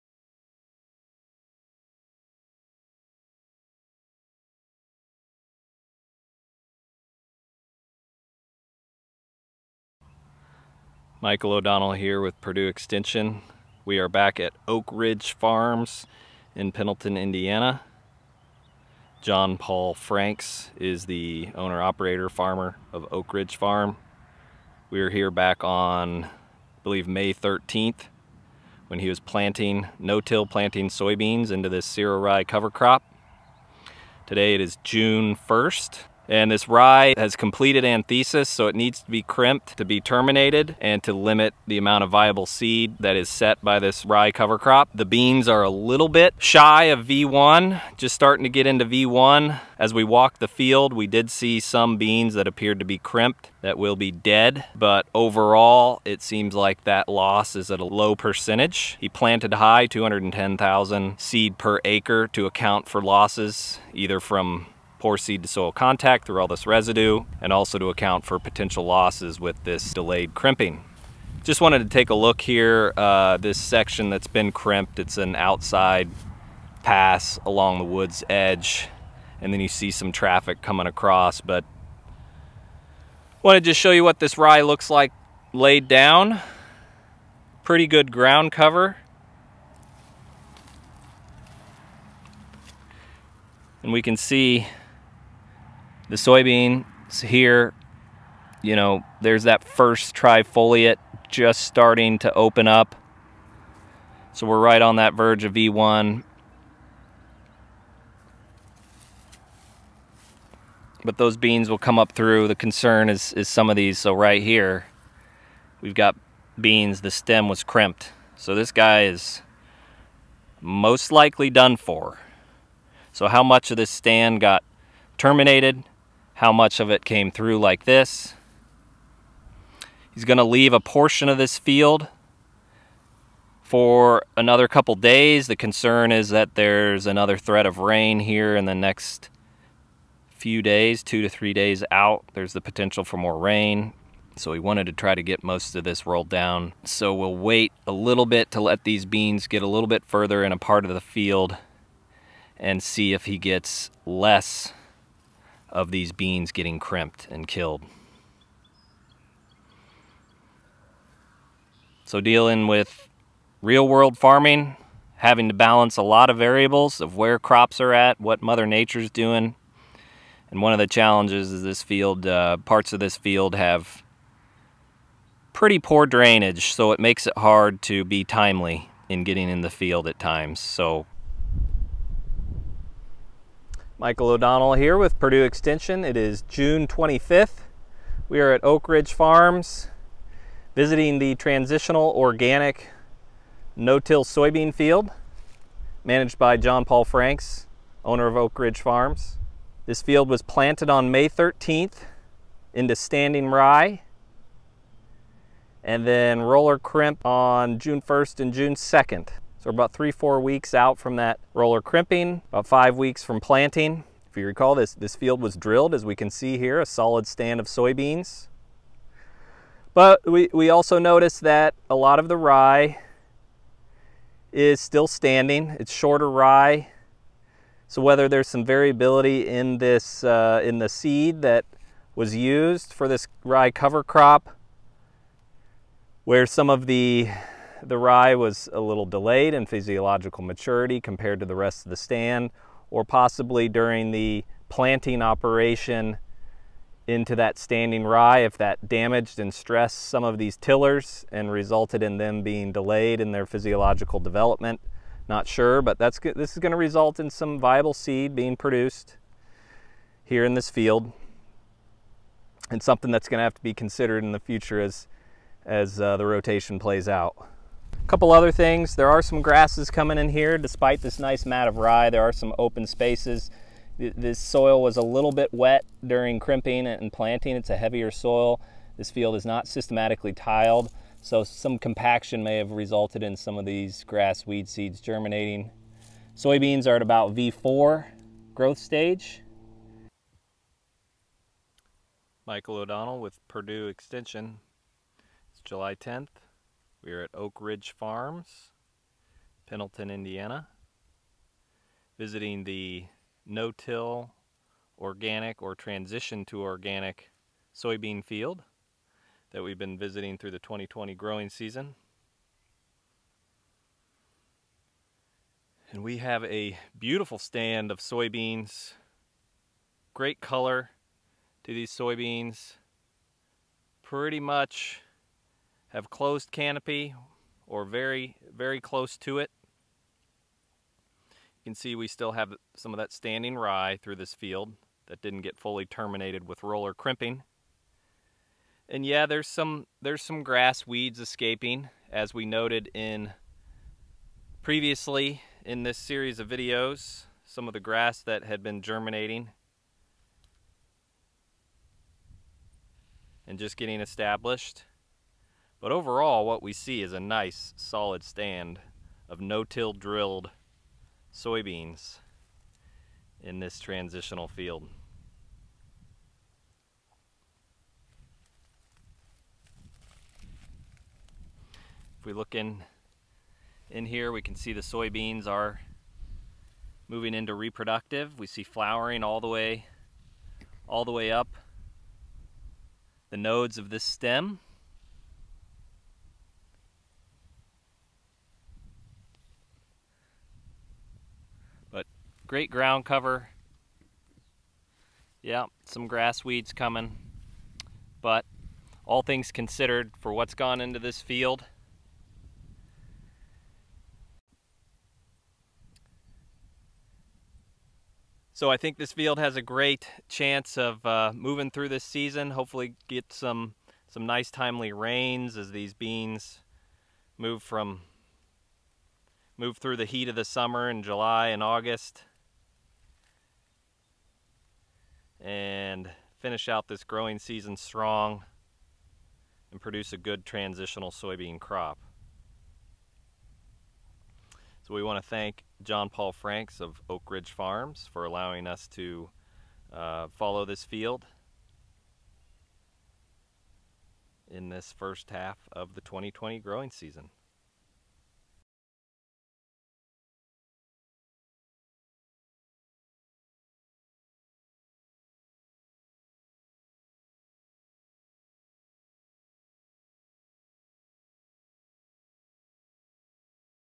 Roller crimp terminating cereal rye cover crop & scouting a transitional organic soybean field
In this video, Purdue Extension Organic & Diversified Agriculture Educator, Michael O’Donnell, revisits a transitional (T2) organic no-till soybean field at Oak Ridge Farms in Pendleton, Indiana, during roller crimp termination of the cereal rye cover crop and tracks soybean crop development through canopy closure.

to learn more about Purdue Extension-Organic Agriculture.